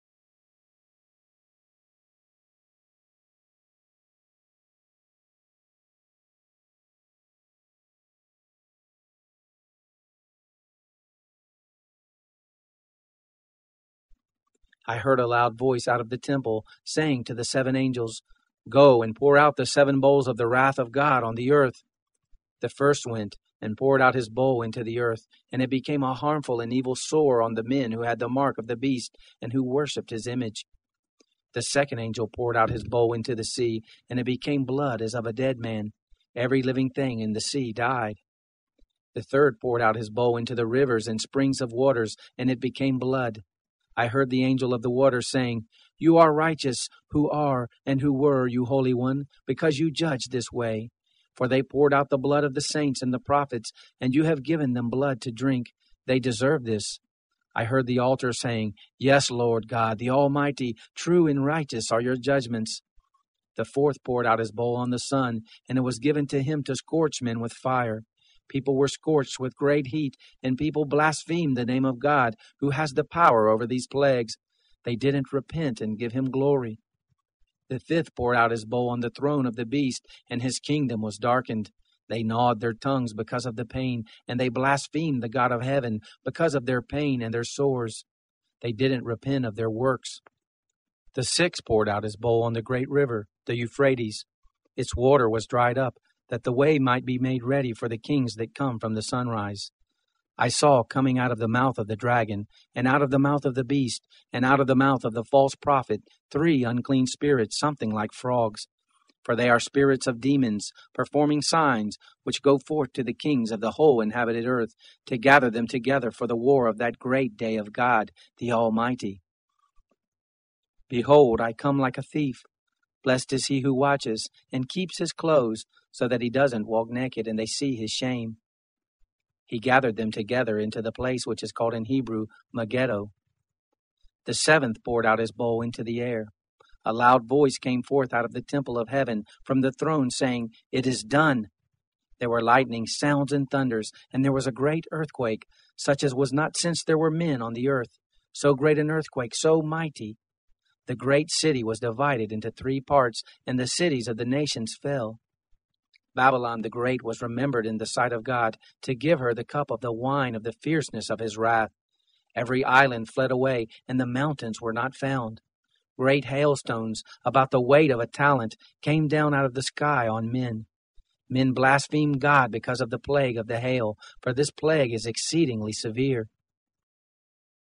Book of Revelation. Chapter 16
Chapter 16 of the Book of Revelation. Read from the the World English Bible version. This is a public domain version based on the 1901 American Standard Version.